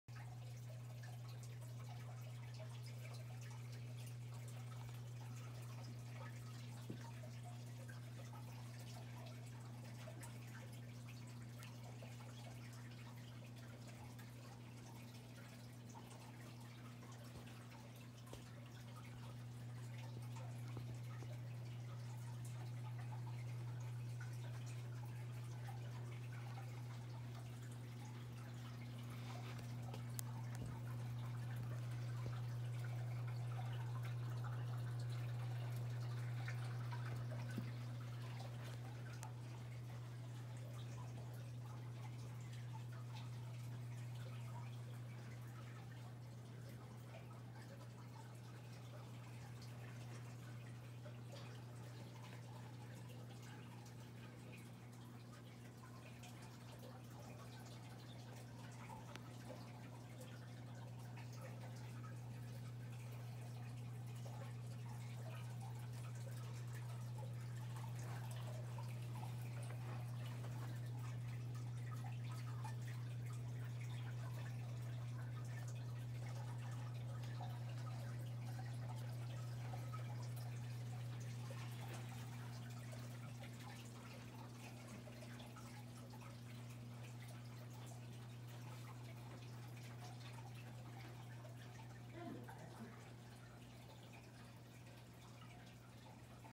My Monster Tank - Arowana, Luteus, Bichir, Oscar, Peacock Bass, Severum
My custom made 8 foot long monster aquarium. Stock includes Arowana, Stage 3 Luteus, Bichir, Oscar, Peacock Bass, Datnoid and Severum.

Has a 125 gallon sump underneath with loads of biomaterial, filter floss, pond filtration pads as well as UV sterililizer.

Thanks to Prime Pets for the custom aquarium and stand.

Thanks to The Waterfront SLC for the majority of the stock.